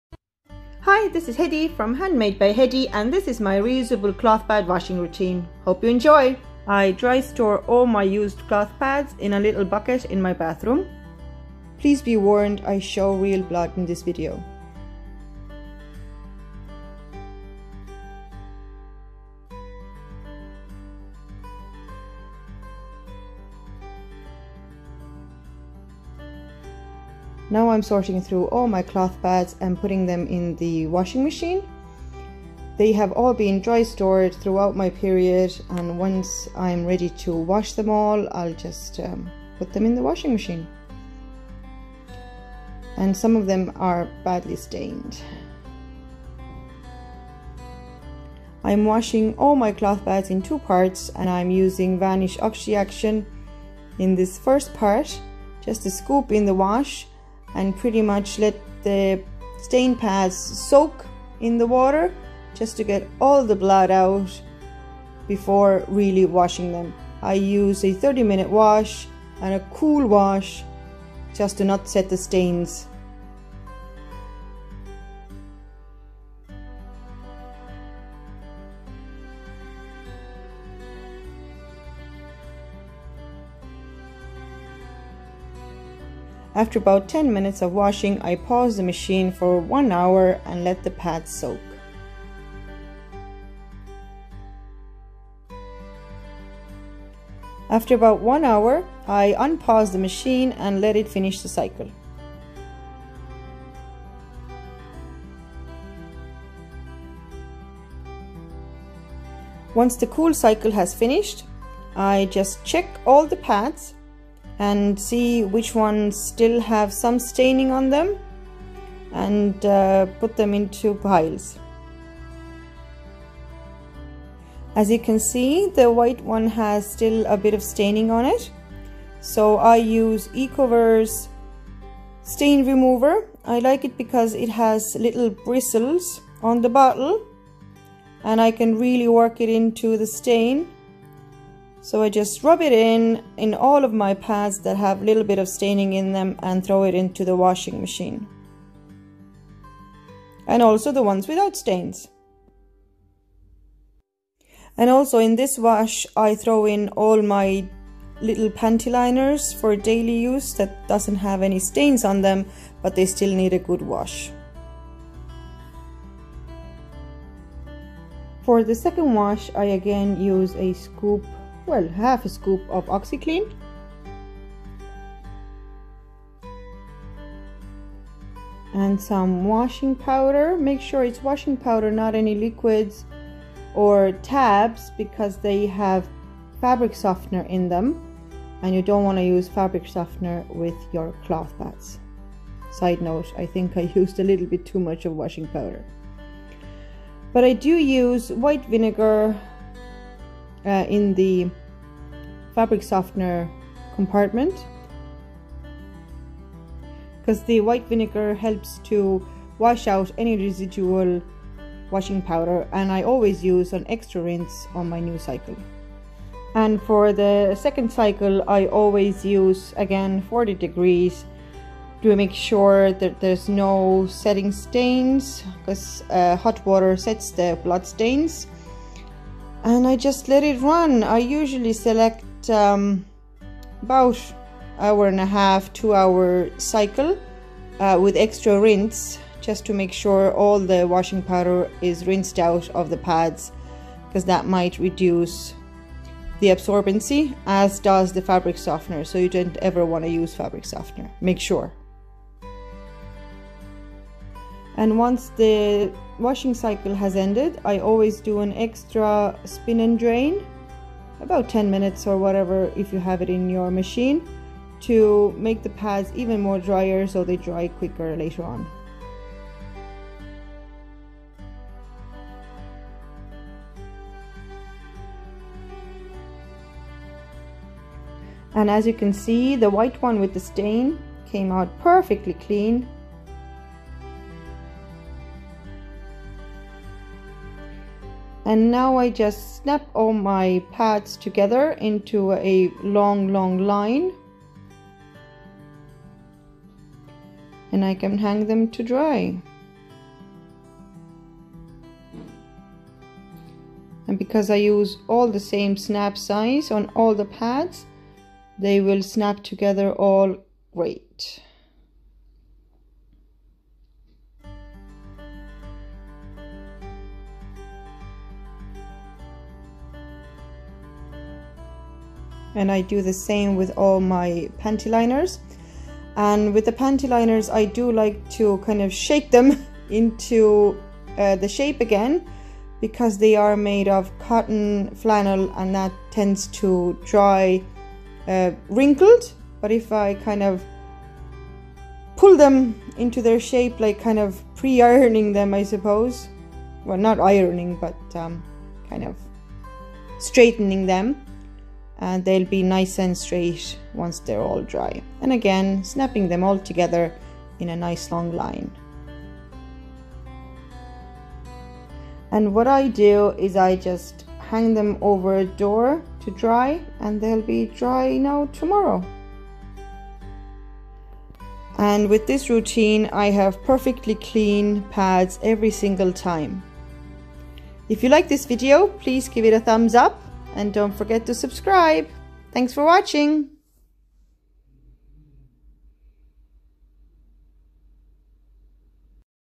Reusable Cloth Sanitary Pads Washing Routine - Quick and Easy, Stain Free Method
A quick and easy method for cleaning your reusable cloth pads. Completely clean pads every time, with no stains. (WARNING! - real blood) More info down below ↓↓↓↓↓↓↓

I always DRY store my used pads in a small bucket in my bathroom and once I'm ready to wash them, I do it in 2 parts. 1st to soak the blood stained ones in a cool wash with oxi clean (I use Vanish) and then I do a proper wash.

If there are any stains still on the pads after the 1st soak I use Ecover stain remover (you can use any you like) and stain treat any pads that need it (just don't be too rough on them) and then just put all or them in the washing machine again (plus all the non trained pads).

I always use 40 degrees wash (hot wash sets blood stains!!!). I use a bit more oxi clean in this 2nd wash plus washing powder(any is fine).

Do not use washing liquid or liquid tabs as most of them have fabric softener in them and you don't want to use ANY fabric softener when washing yous cloth pads as this will reduce their absorbency and then the pads are pretty much useless.

I do how ever use white vinegar, to wash out any residual washing powder (and I always do an extra rinse on my second wash and an extra spin)

Once the pads are washed I just snap them together and hang to dry.

Also please visit my etsy shop HandmadeByHedi.etsy.com